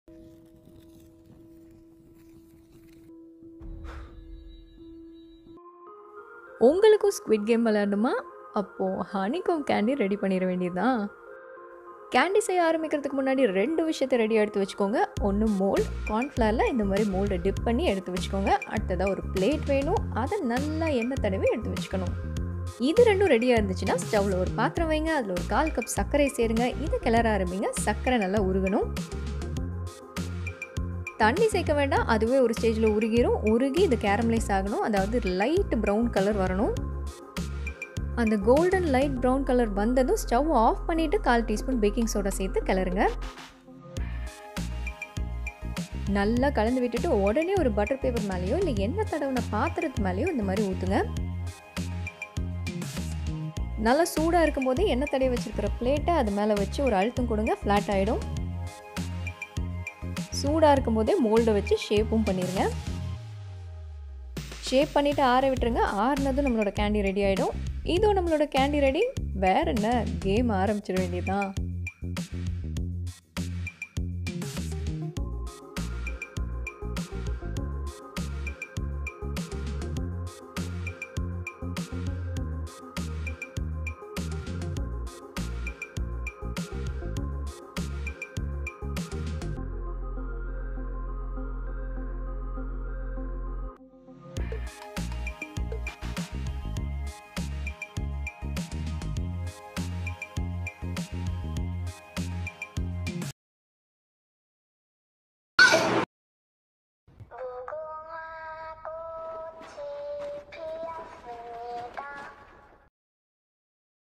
நாங்களும் பண்றோம் squid game challenge | Squid game candy | Honeycomb candy | Sweets recipe in tamil
நாங்களும் பண்றோம் squid game challenge | Squid game candy | Honeycomb candy | Sweets recipe in tamil | korean candy | korean dalgona candy |  Trending dalgona candy

▶️INGREDIENTS FOR DALGONA CANDY:
Sugar - 1/4 cup
Cornflour- 1 tbsp for dusting